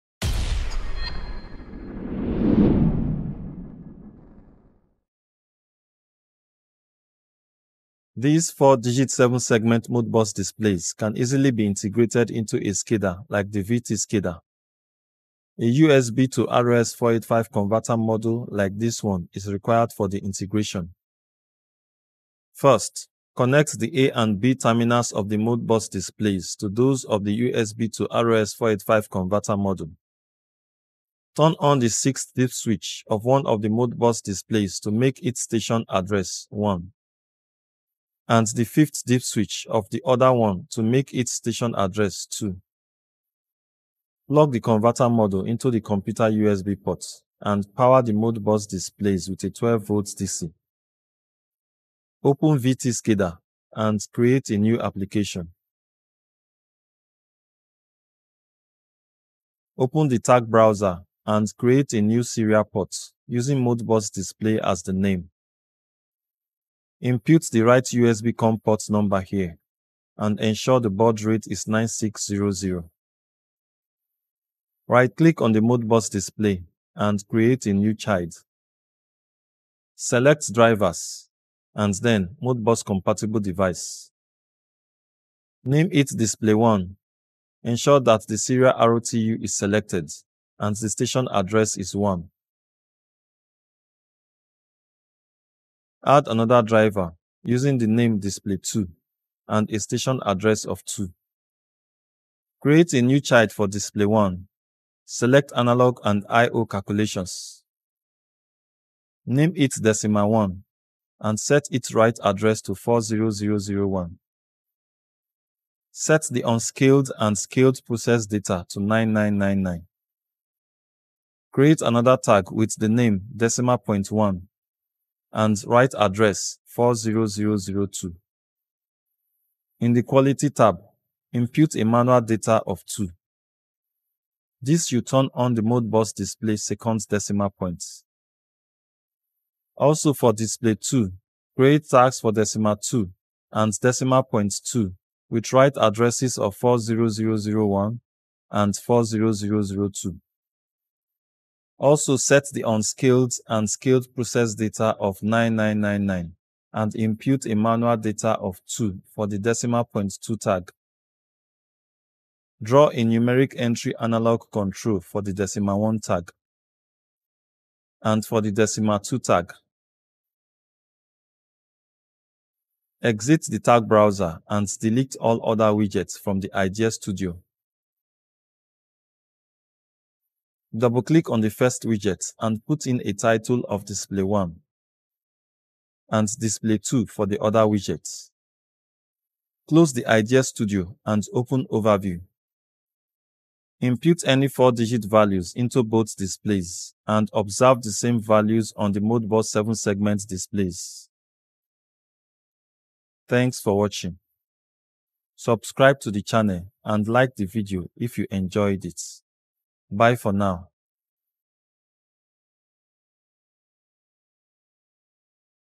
SCADA Modbus 7 Segment Display
The Modbus communication protocol allows PLCs and other Modbus-compatible devices, like the 7-segment Modbus display, to exchange data with SCADA systems. The 7-segment Modbus display supports the Modbus serial or RTU implementation. The video demonstrates the configuration of VT SCADA communication through the USB-to-RS485 module Modbus RTU communication protocol to display data on the 7-segment display module.

The Arduino Nano PLC built by J R Electrokits is based on the open-source hardware Outseal PLC Nano, an Outseal automation technology brand.

Support videos on this channel when you buy products from AliExpress using the below link(s).
1. Industrial USB To RS485 Converter Module